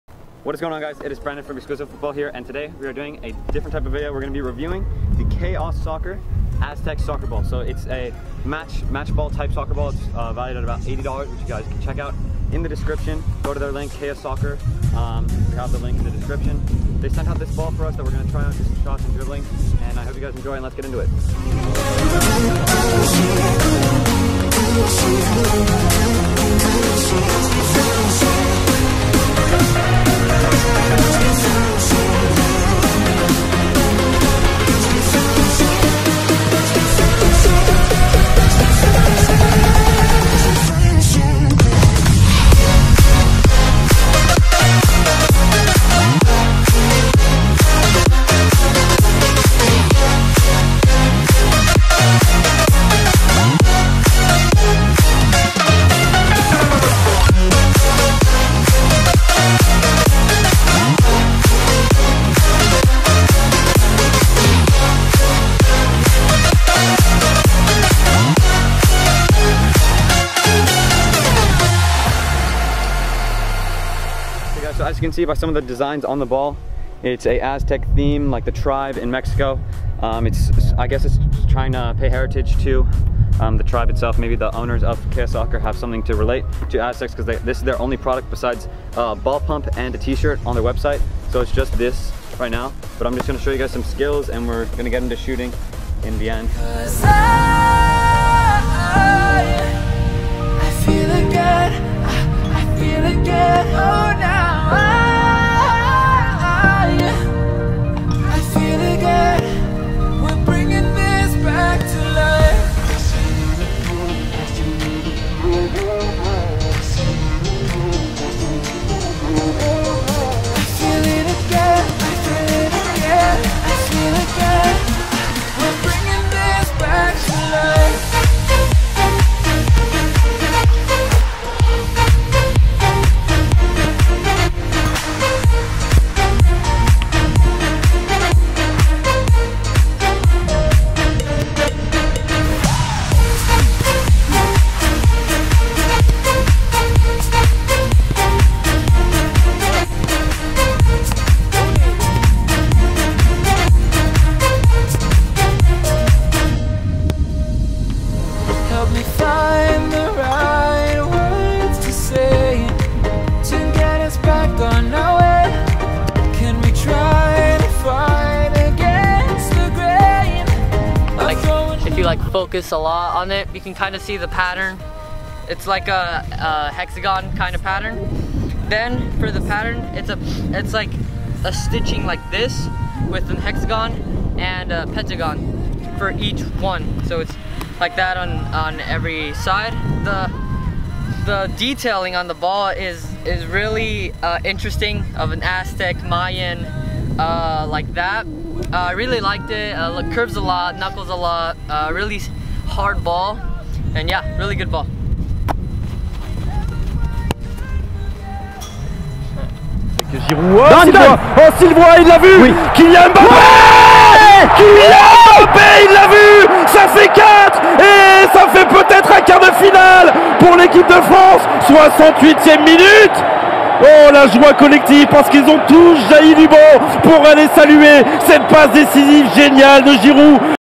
UNBOXING Aztec Soccer Ball!
Today we head to the field to unbox the Aztec Soccer Ball from Chaos Soccer Gear!

CAMERA GEAR DOWN BELOW

- NEW CAMERA
- OLD CAMERA
- SHOTGUN MIC
- BENDY TRIPOD
- GAME CAPTURE
- CAMERA GLIDER